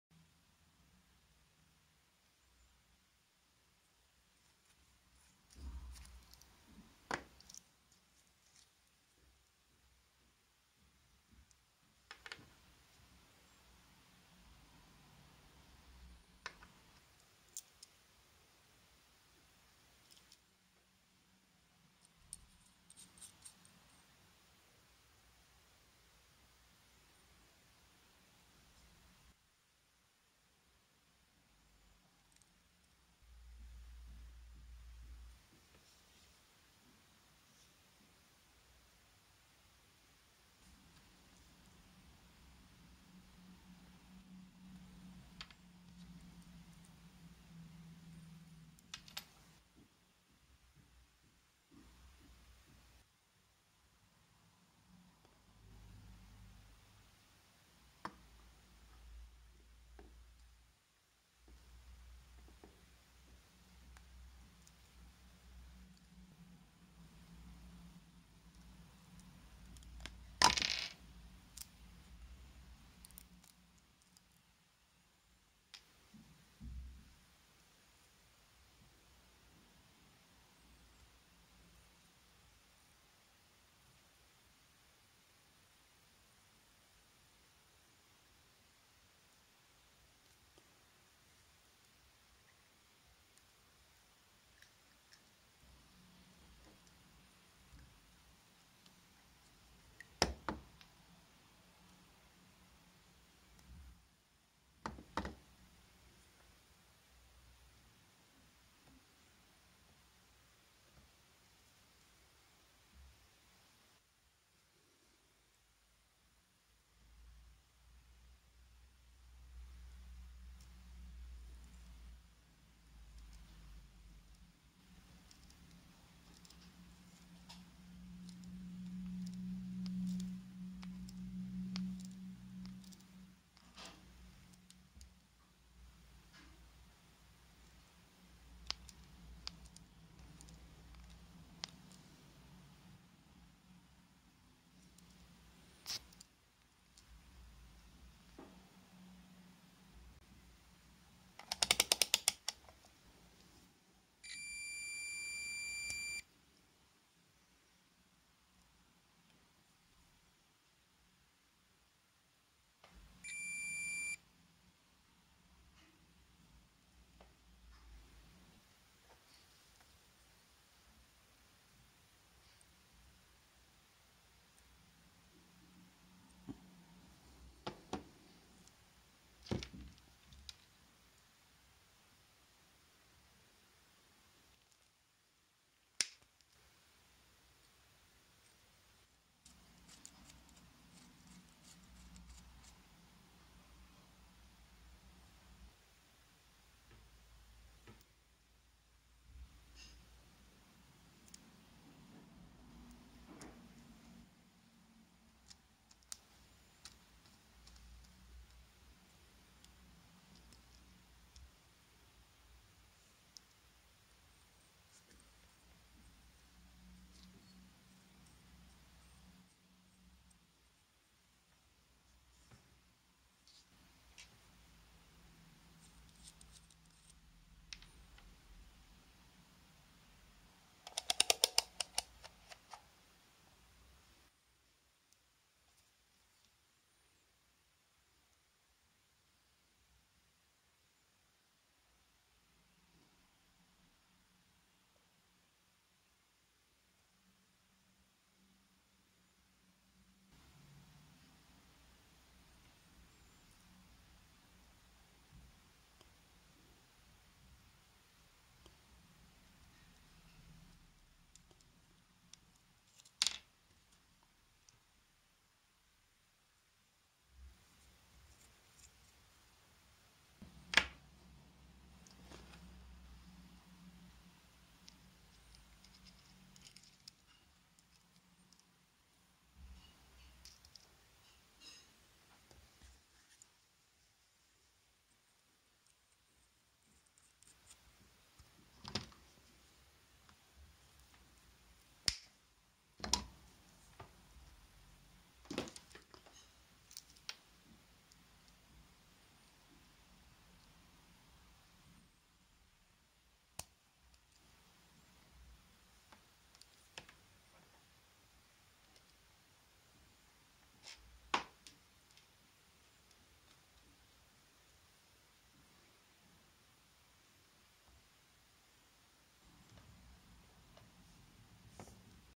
2 Way Splitter on Coaxial Cable with F Connectors
Watch how to install a two way splitter on coaxial cable using F connectors, how to  joint antenna cable and put on 75 ohm terminal on empty slot.

1. F connector installation to 75 ohm coaxial cable
2. Two way splitter on coaxial cable
3. TV antenna connector
4. F connector tutorial for coaxial cable
5. 75 ohm terminal